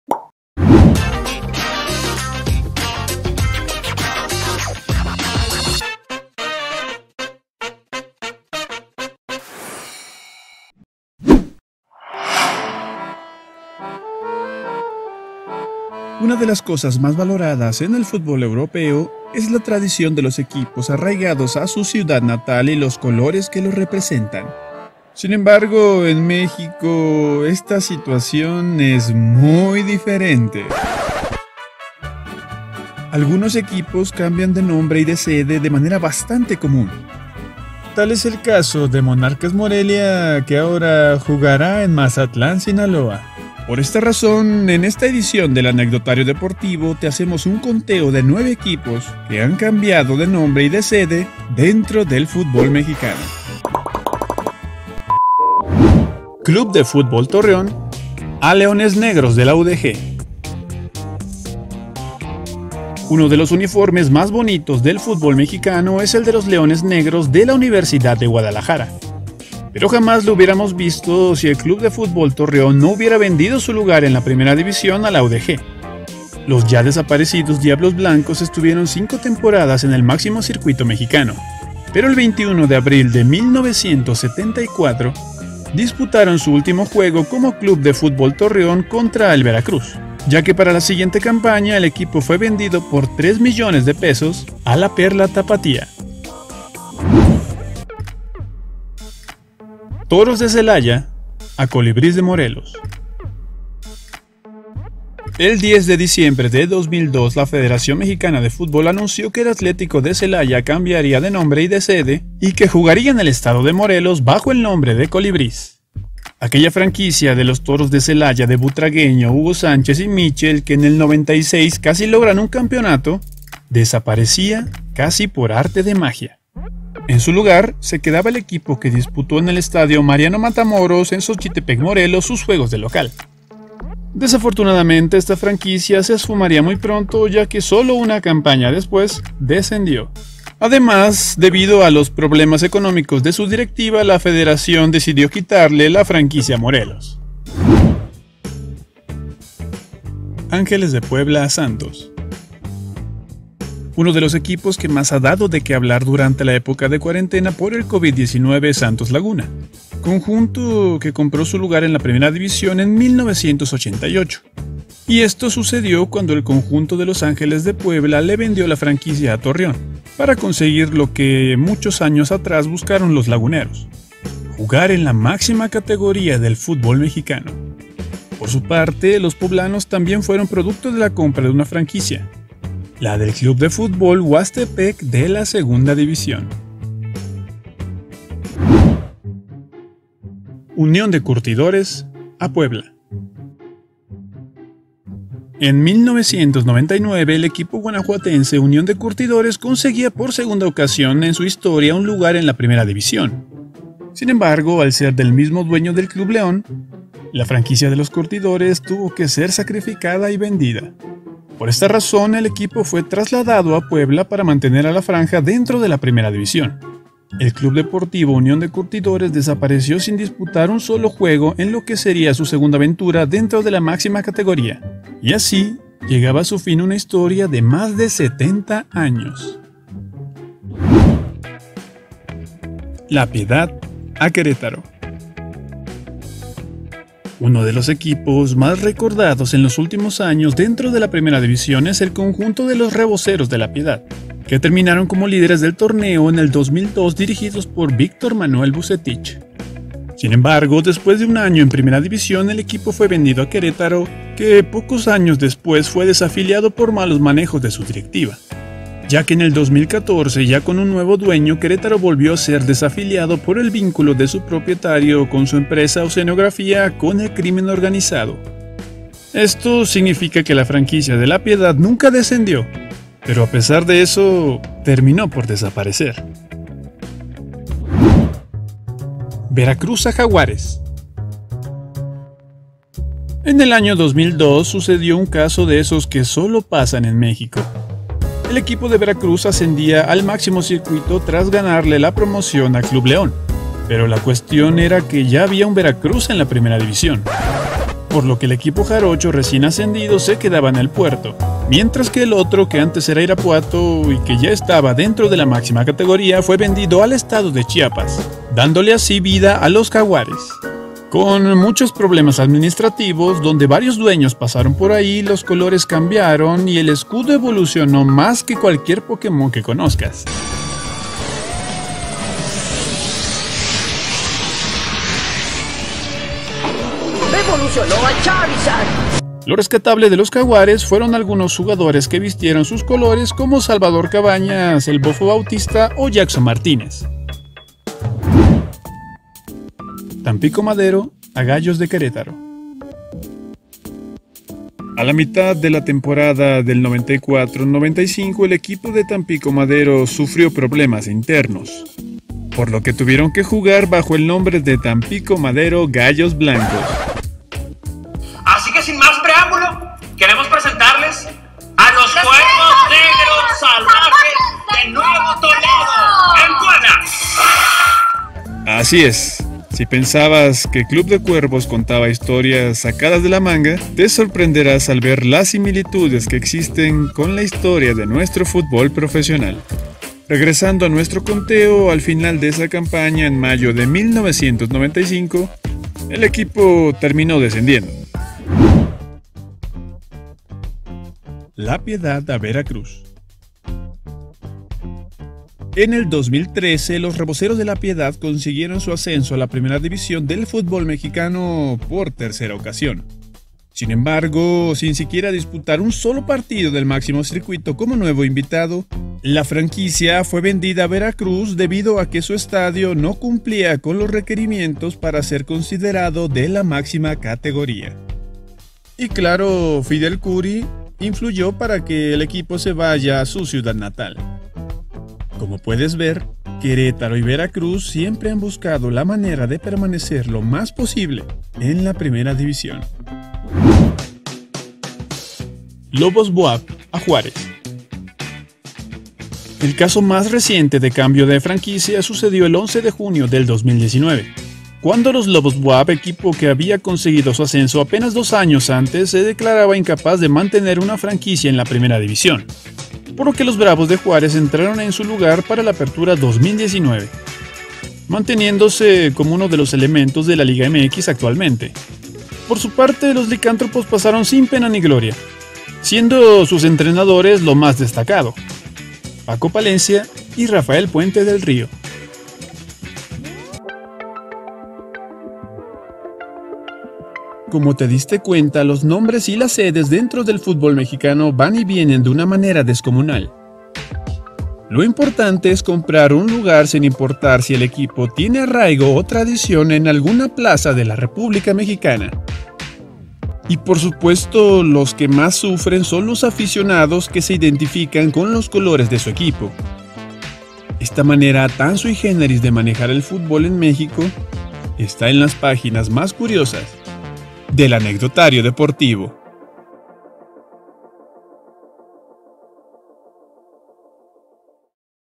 Equipos que desaparecieron de la Liga MX 😭| Anecdotario Deportivo
Monarcas Morelia anunció de manera oficial el 2 de junio de 2020 su solicitud a la LIGA MX para cambio de SEDE y de NOMBRE.

Es por eso que en este conteo del AnecdotarioDeportivo te contamos sobre otros equipos que, como Monarcas Morelia, cambiaron de sede, y de nombre. Algunos haciéndolo más de una vez.

No cabe duda que en el fútbol mexicano, muchas veces se deja de lado el arraigo y el cariño de los aficionados, que importan poco cuando hay bastante dinero de por medio.